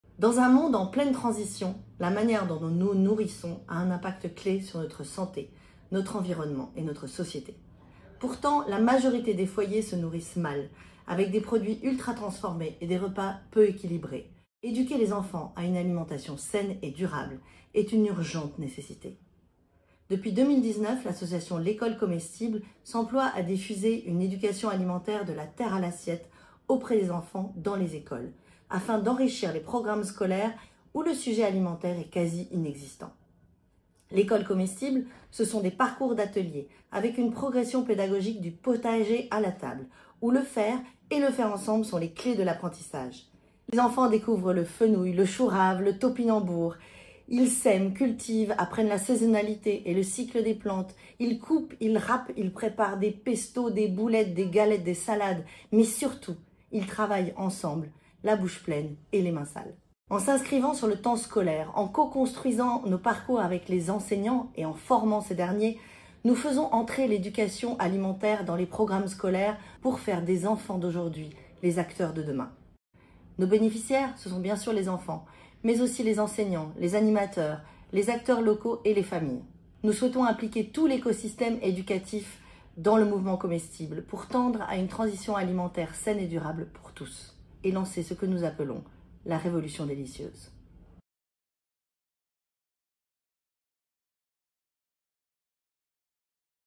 Projet de l'association L'Ecole Comestible - Rencontres pour la Planète 2023
L'association l'Ecole Comestible nous présente son projet "Education à l’alimentation durable dans les écoles" pour lequel elle recherche des fonds à l'occasion des Rencontres pour la Planète 2023.

L'Ecole Comestible a pour missions d'éduquer les enfants à une alimentation saine et durable, de la terre à l'assiette, pour les rendre acteurs de la transition alimentaire